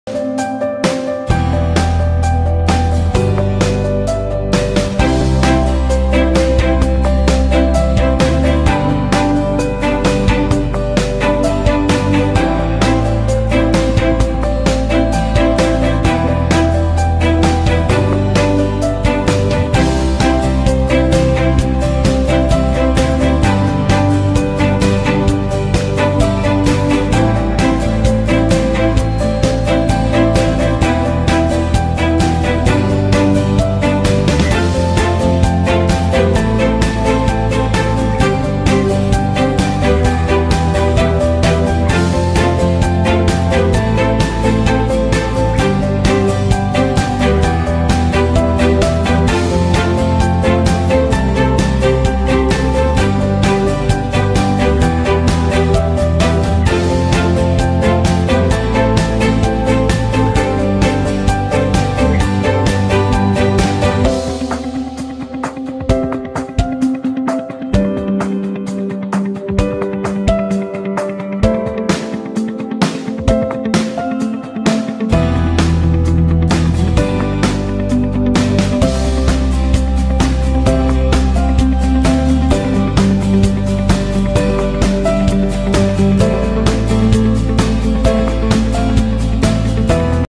Bedbug Heat Treatment explained Info video animation by Rapid Pest Control.
Rapid pest control is quality pest control service providers in London Ontario and surrounding towns we are bedbugs control specialists.